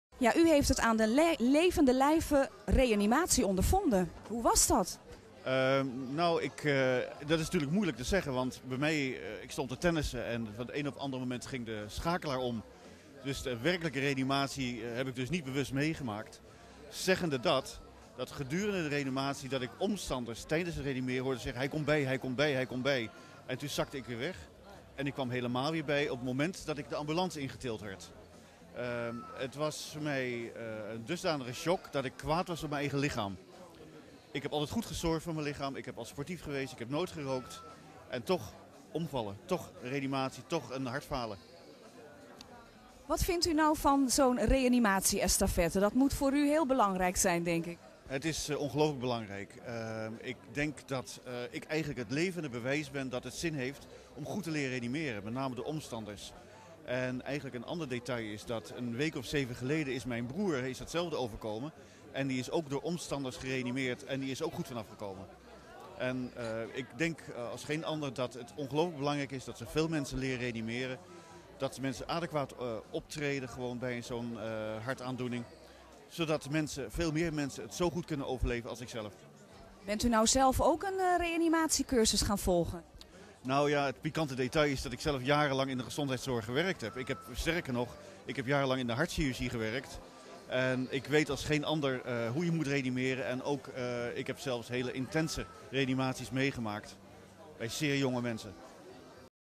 De heer W. Snoeks, overlever
Interview met Wim Snoeks, overlever